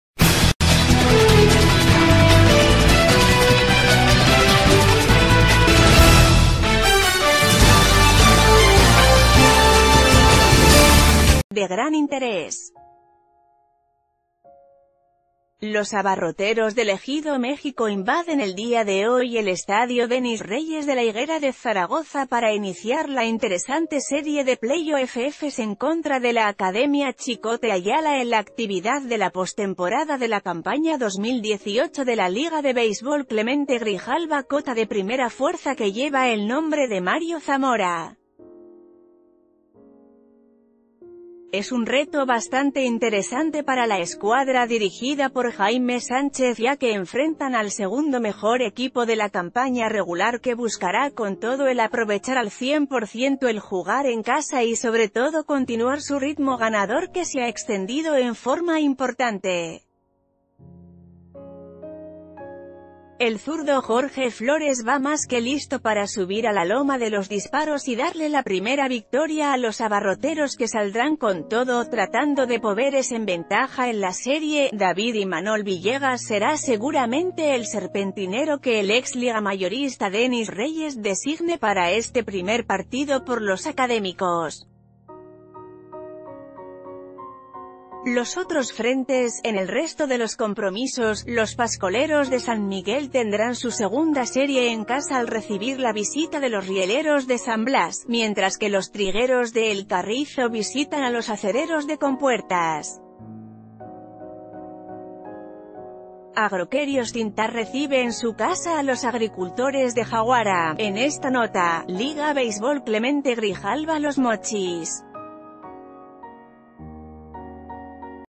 A parar a la Academia
De gran interés. Los Abarroteros del ejido México invaden el día de hoy el estadio Dennis Reyes de la Higuera de Zaragoza para iniciar la interesante serie de play-offs en contra de  la Academia Chikote Ayala en la actividad de la postemporada de la campaña 2018 de la Liga de Beisbol Clemente Grijal...